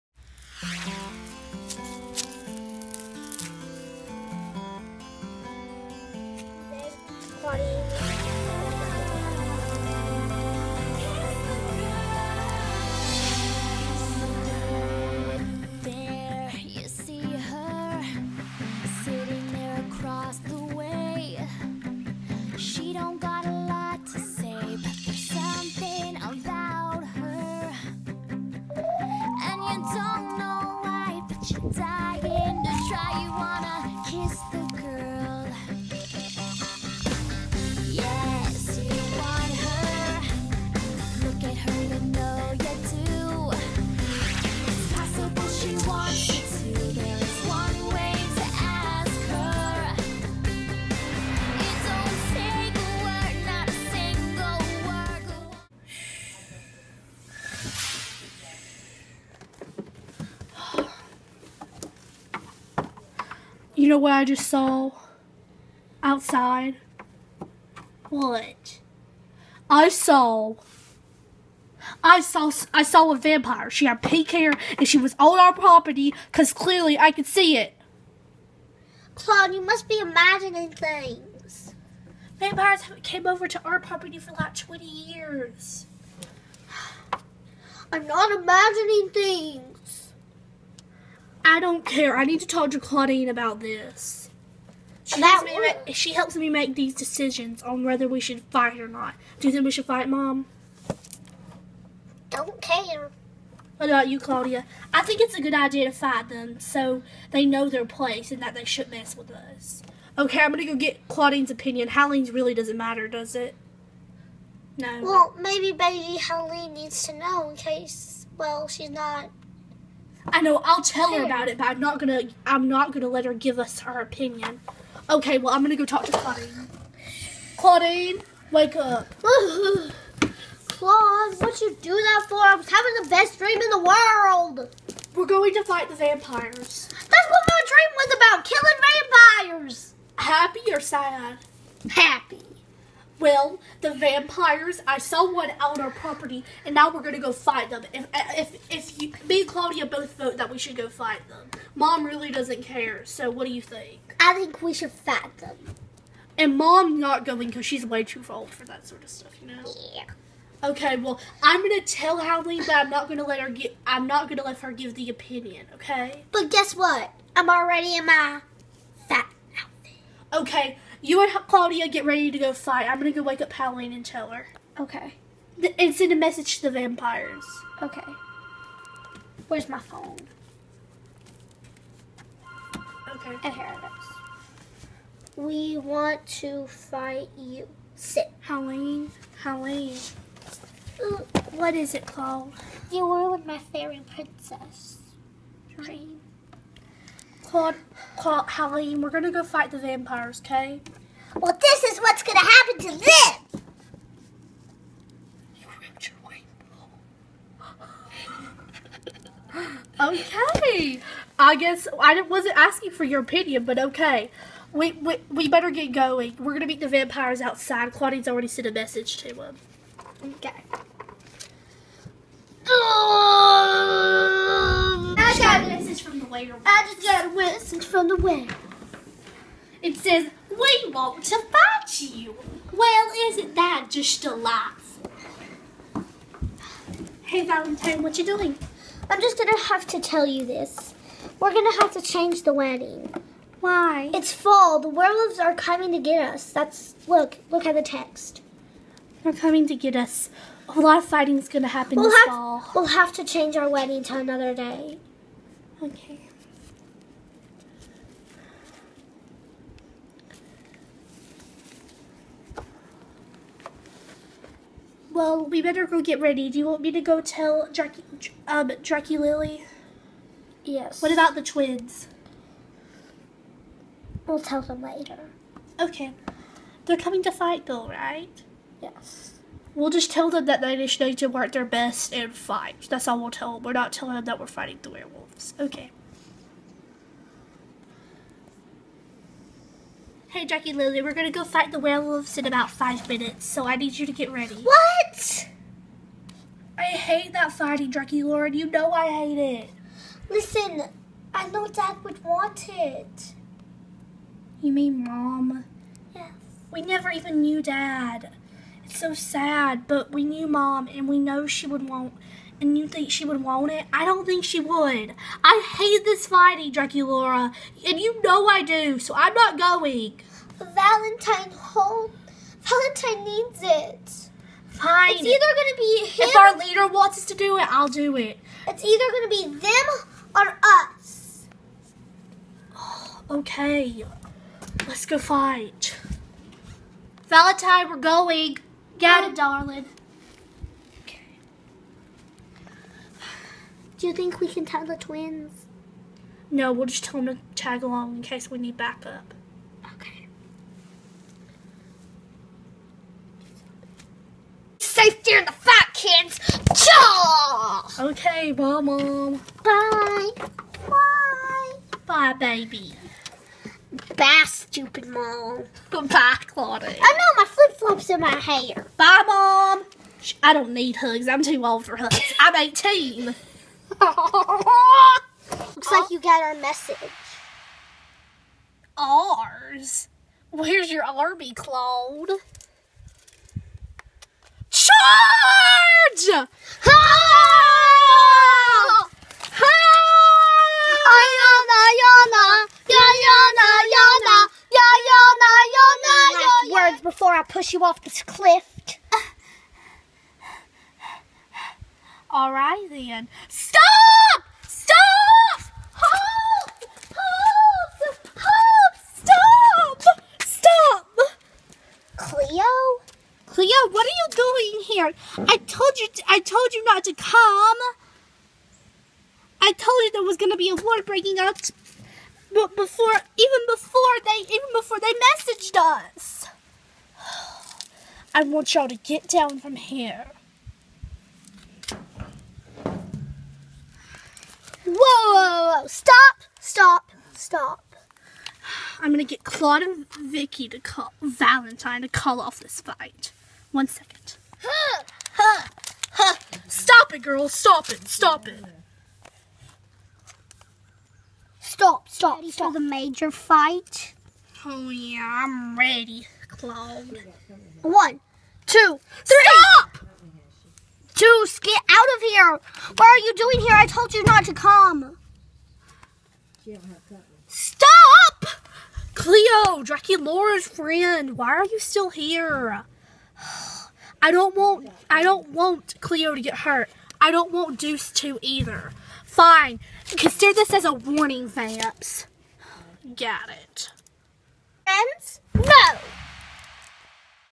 Fangs and Fur Season 1 Episode 1 "The First War"
Fangs and Fur is a new TV series on the BigSisLittleSis Dollhunters channel. Fangs and Fur is about a group of werewolves and vampires who fight in war and there are occasionally romantic moments! If you want a mix of undying love and and epic battles then Fangs and Fur is the right show for you!